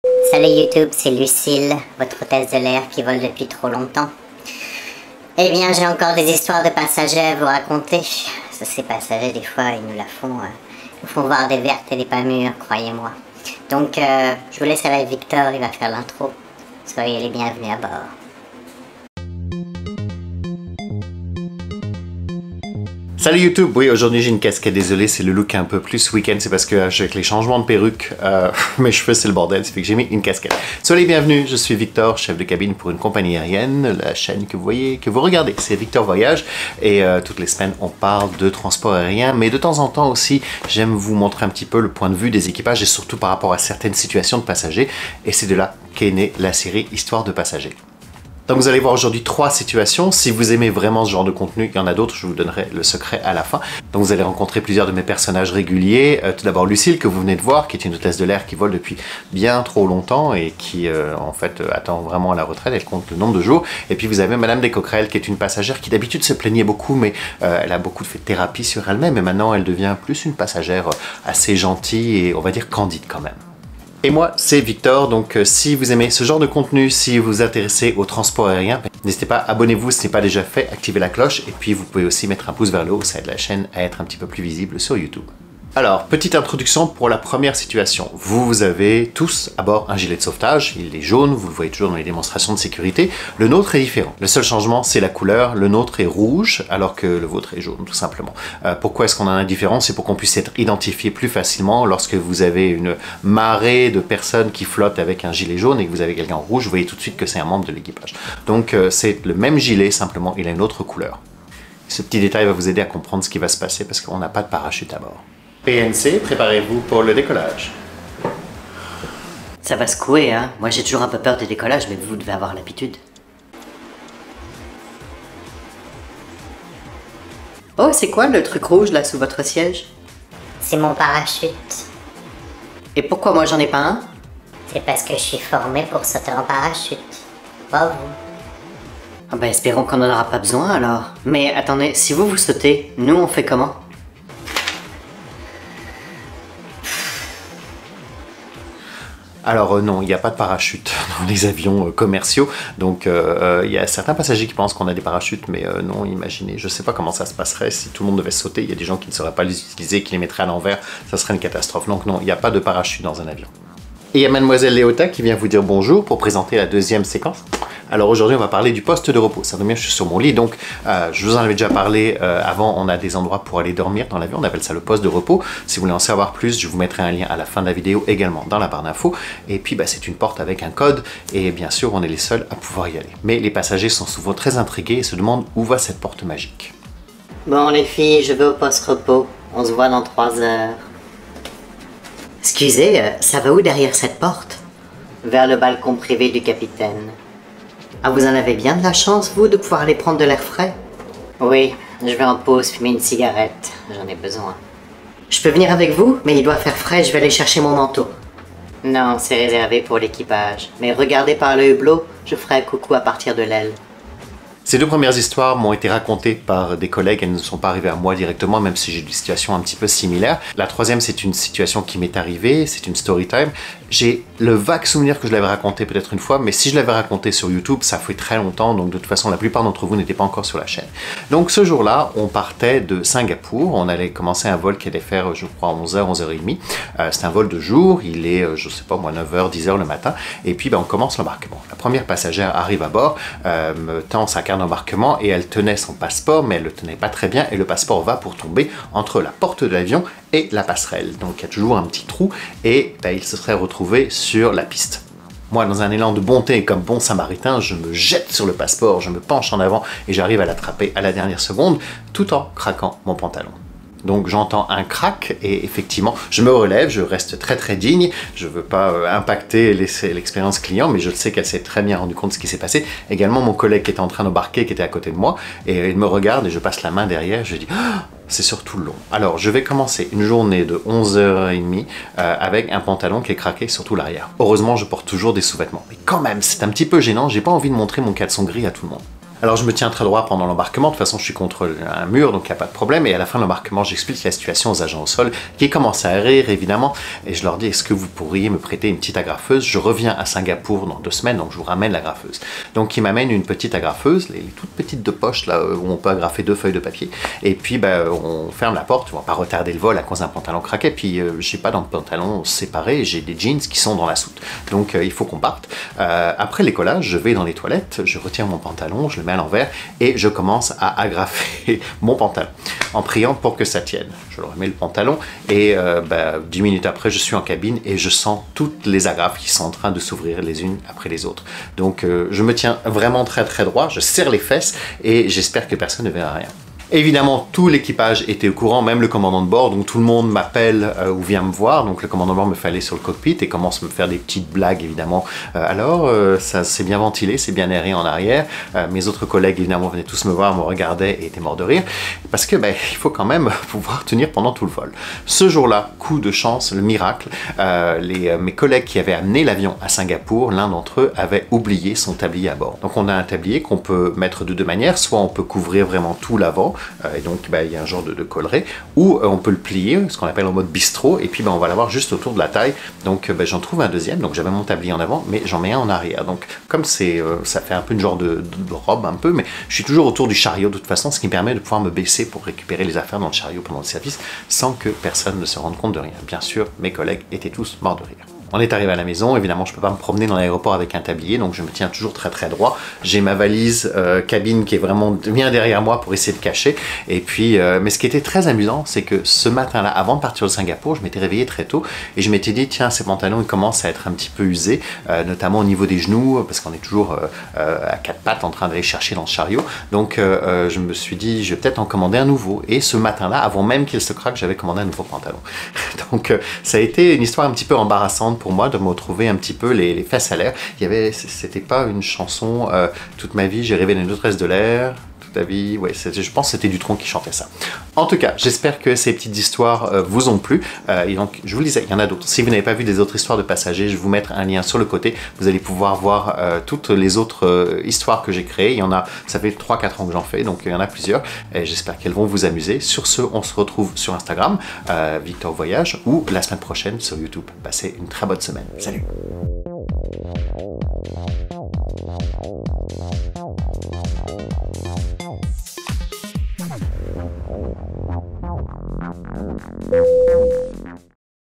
Histoires De Passagers: PARACHUTES | Victor Voyage
Lucielle et Mme Descoquelles sont de retour pour de nouvelles aventures
On parle de parachutes et du balcon privé du pilote. En bonus, je vous raconte un moment très embarrassant

Time Code:
0:00 Intro
1:25 Le parachute
2:43 Le balcon du pilote
3:35 Le pantalon de Victor